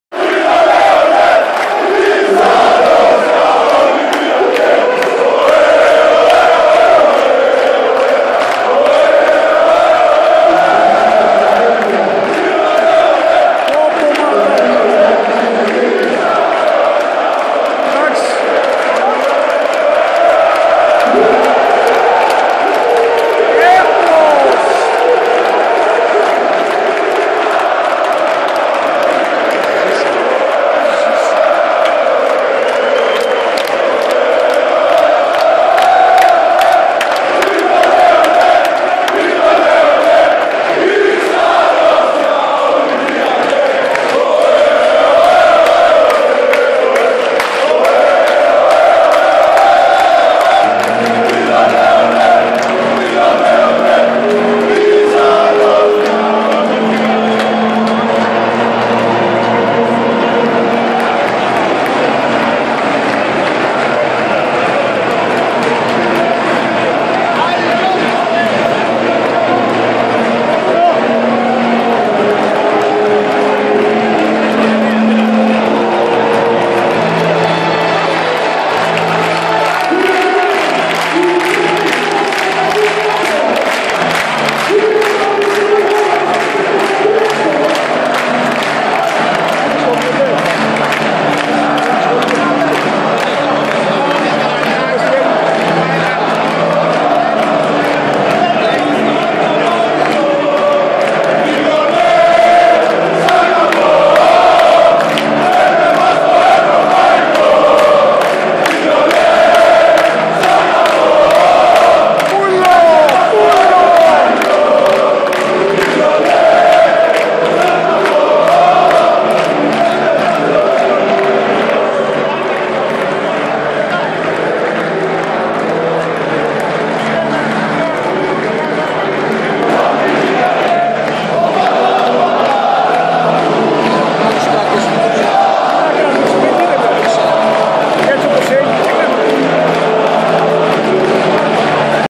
Ολυμπιακος - Barcelona 71 - 68
Πανηγυρισμοι μετα το επικο buzzer beater του Πριντεζη !